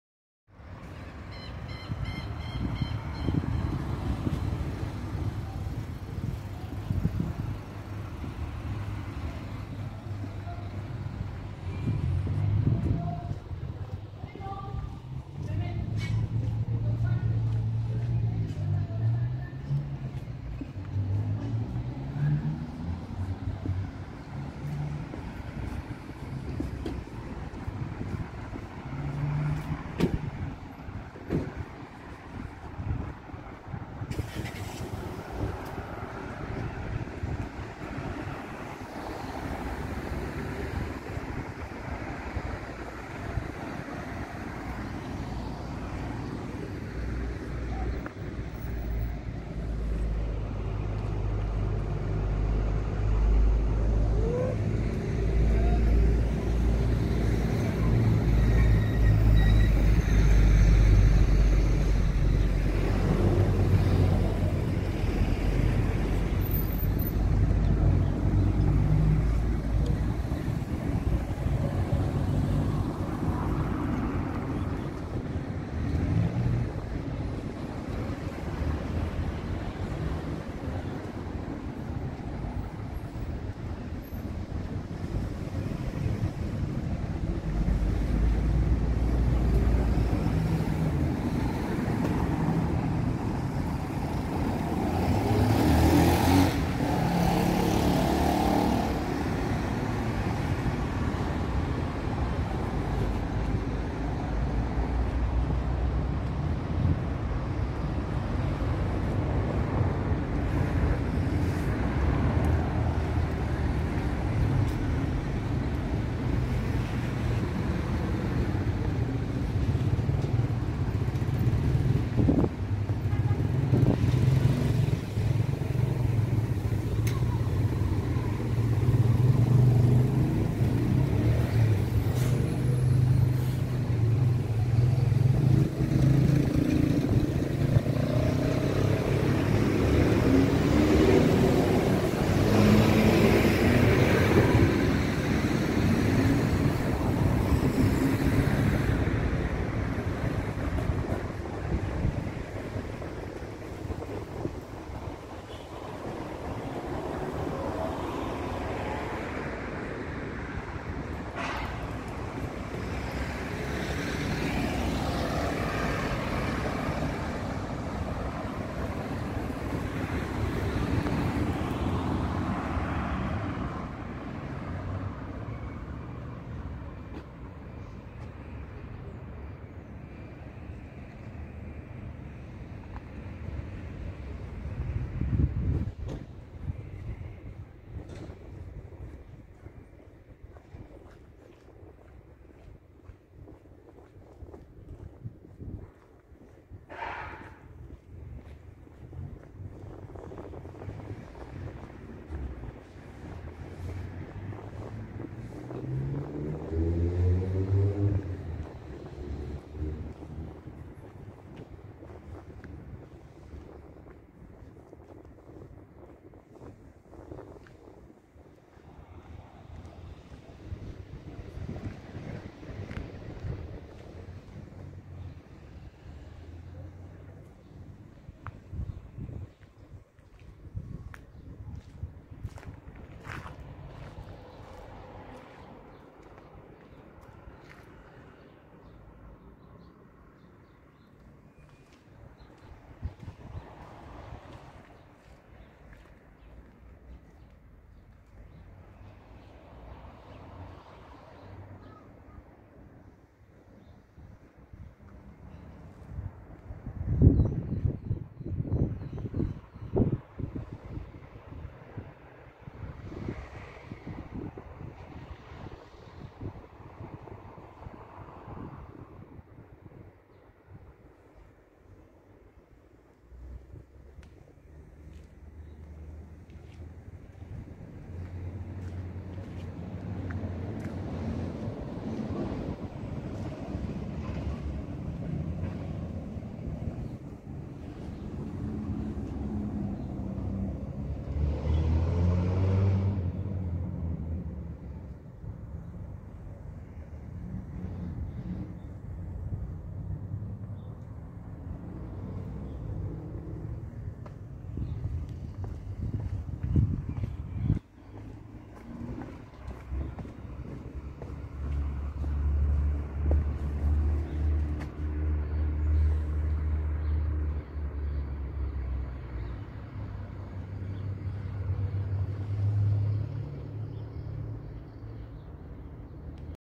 Walking tour otros barrios~ Bariloche Agosto 2021
Caminamos por otra calle comerciales de Bariloche que se encuentran mas arriba de las turísticas conocidas, son donde nos movemos los barilochenses.